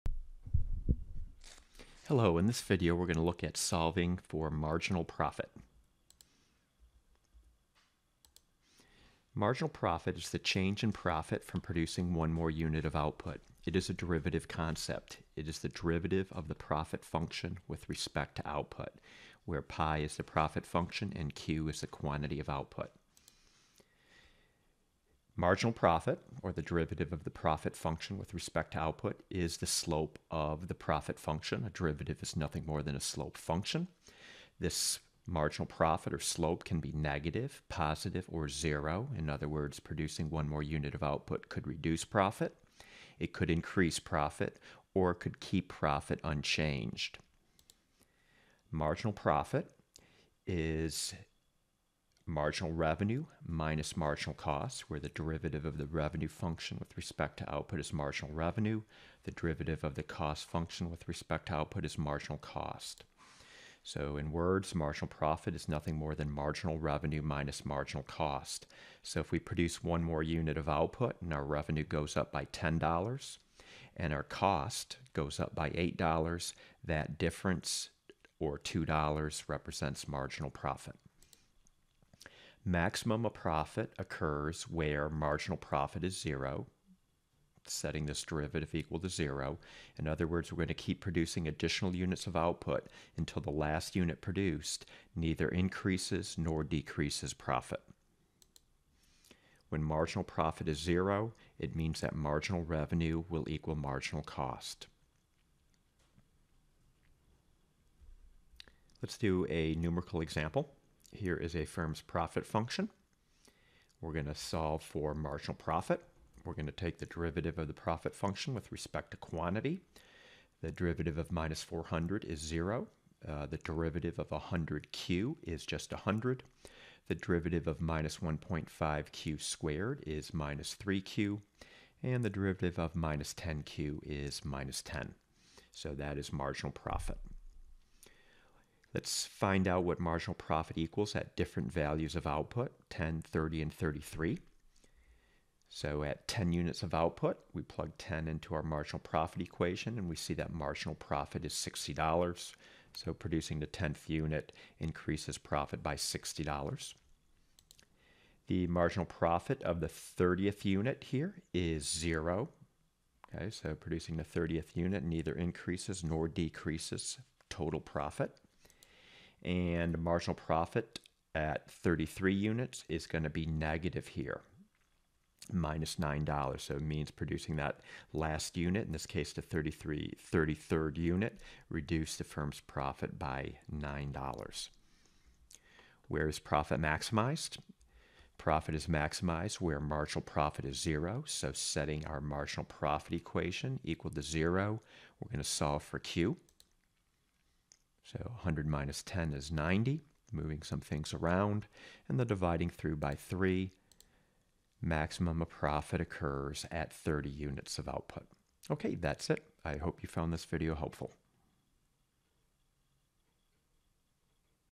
Solving for Marginal Profit
What is marginal profit and how to solve for it with a numerical example.